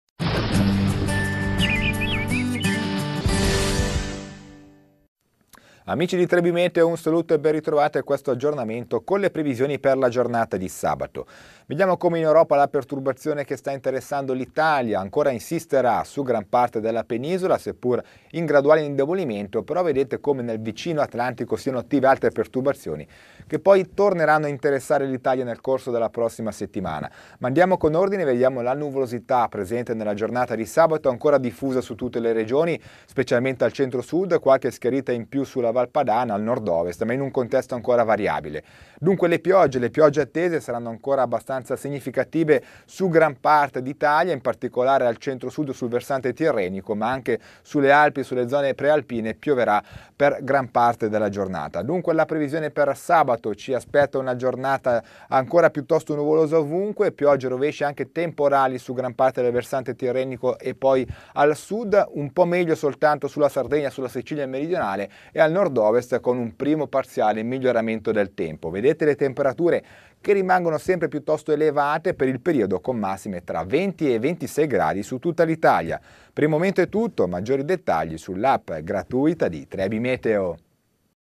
Previsioni meteo Video per sabato, 21 ottobre
Previsioni del tempo a cura di Daniele Berlusconi Per aggiornamenti in tempo reale visita